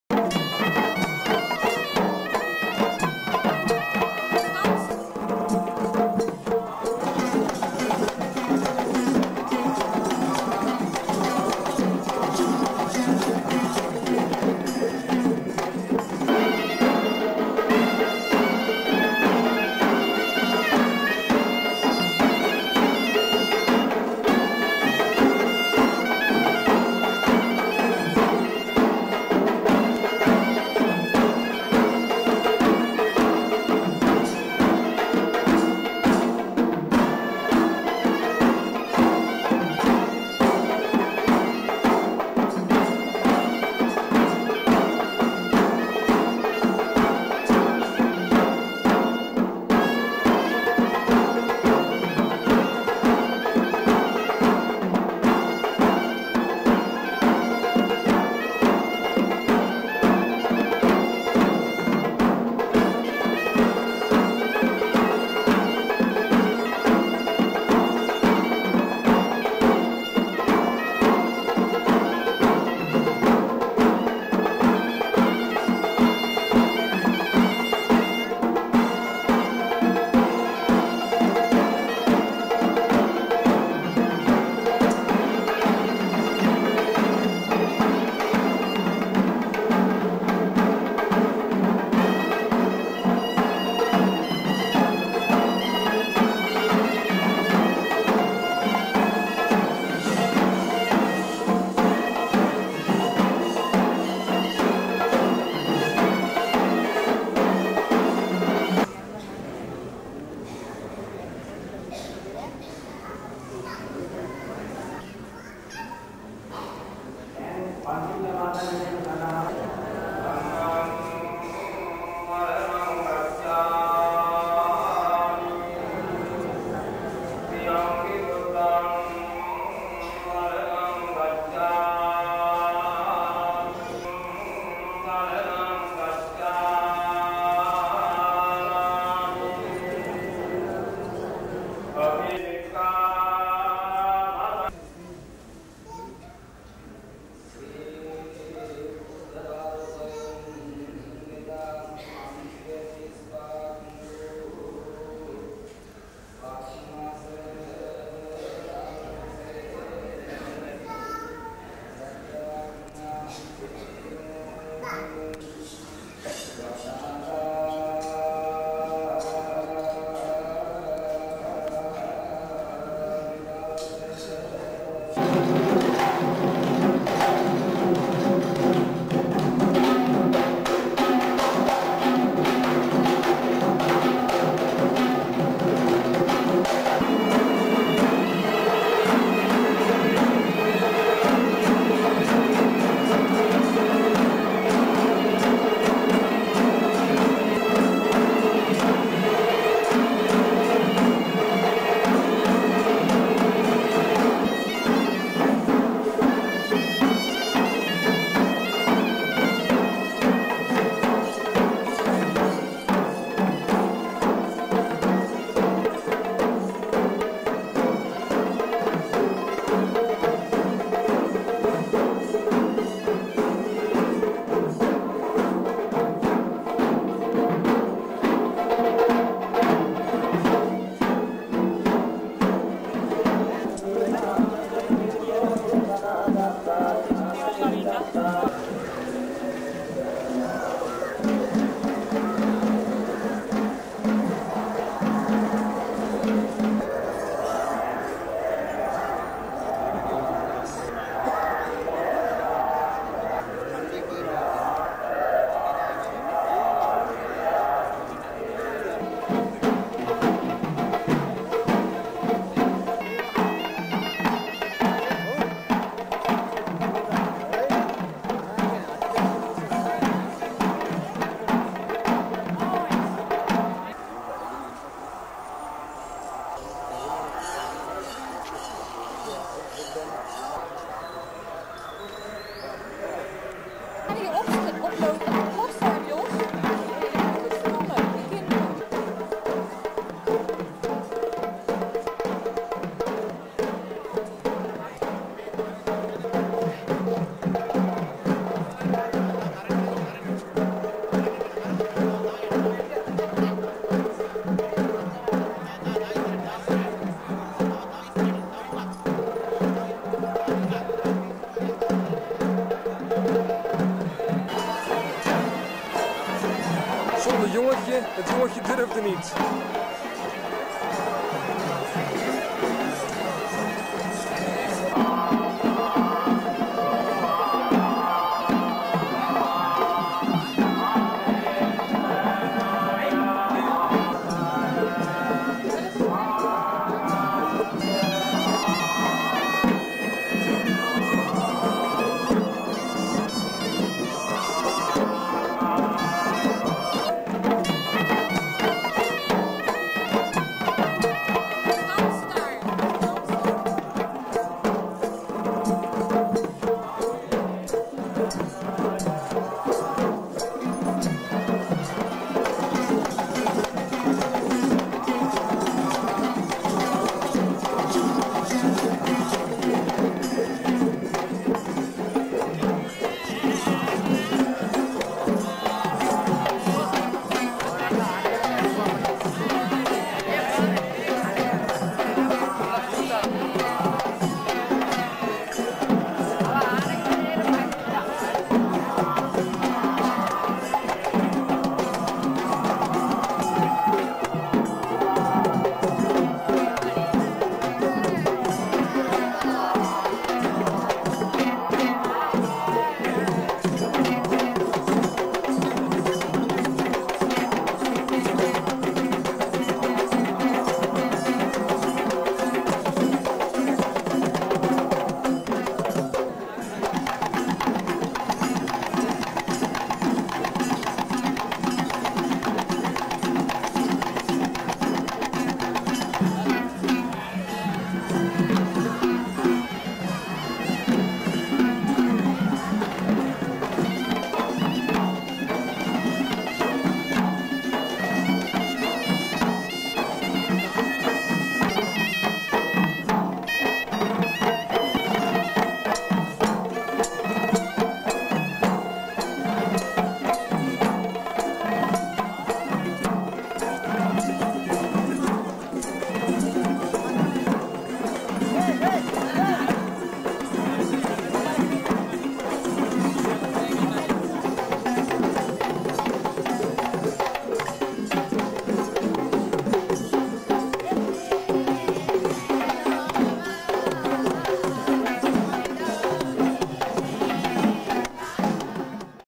Kandy dancers - Unique performance in the streets of Kandy (1992)
Ceremony with monks and Kandy dancers in the temple of the tooth in Kandy (1992, Sri Lanka), followed by a procession where the Kandy dancers performed beautiful in the streets of Kandy.